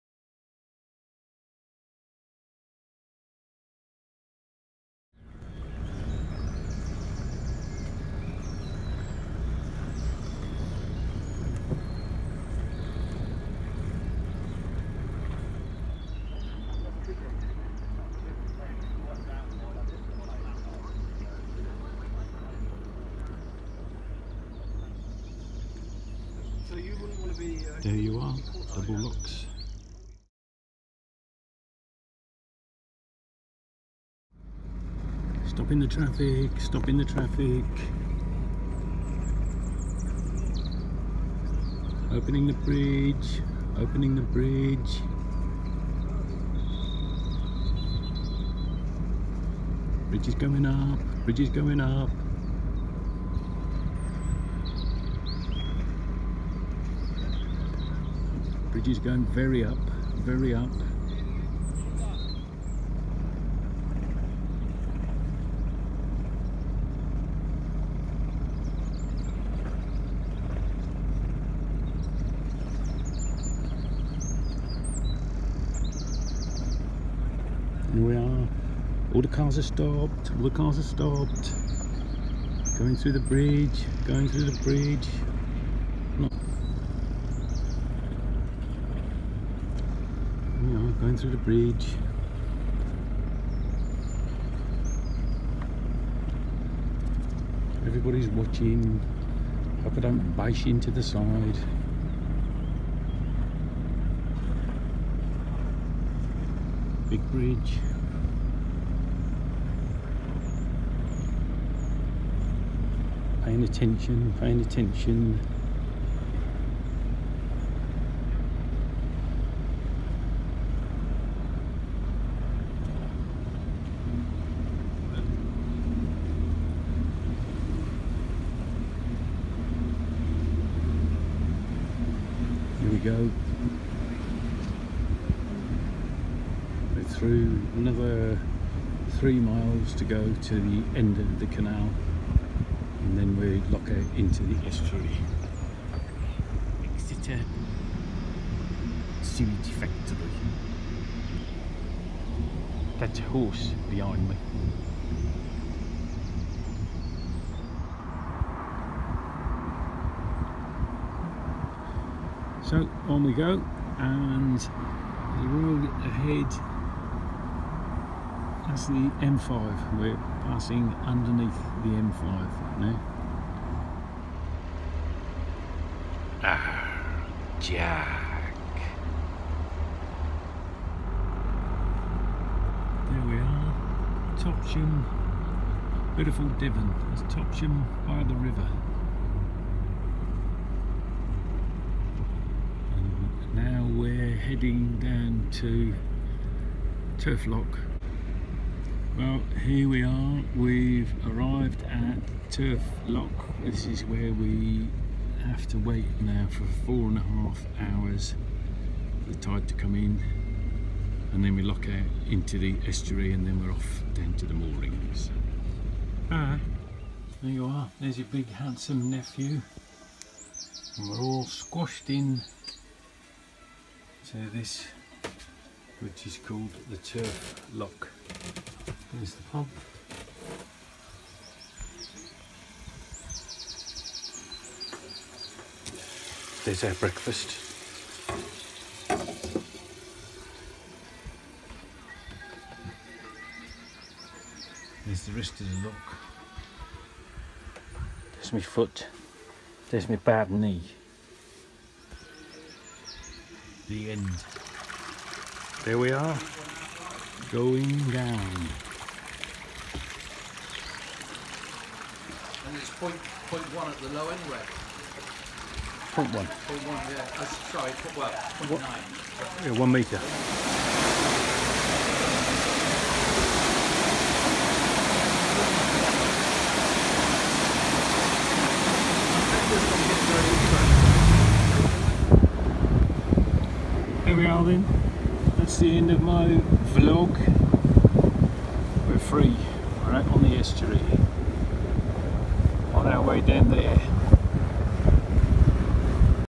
Exeter Quay to Exe Estuary through Turf lock.
Bill and Wills escape from lockdown June 1st 2020